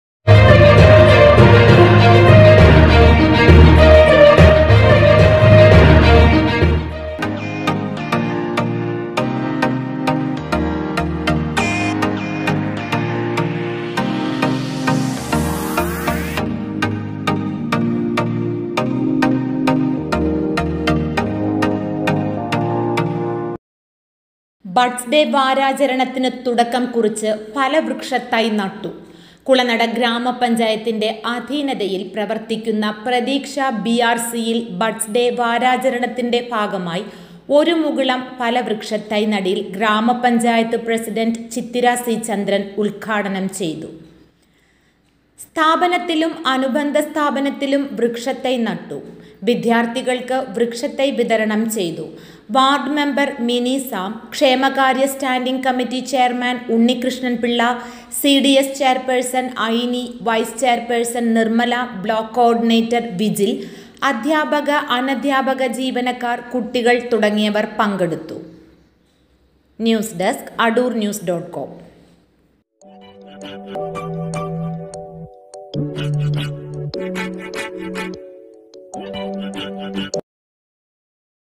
'ഒരു മുകുളം' ഫലവൃക്ഷ തൈ നടീൽ പ്രതീക്ഷയേകുന്നു@ADURNEWS
കുളനട പ്രതീക്ഷ ബഡ്സ് റിഹാബിലിറ്റേഷൻ സെന്ററിൽ ബഡ്സ് ഡേ വാരാചരണം തുടങ്ങി@ADURNEWS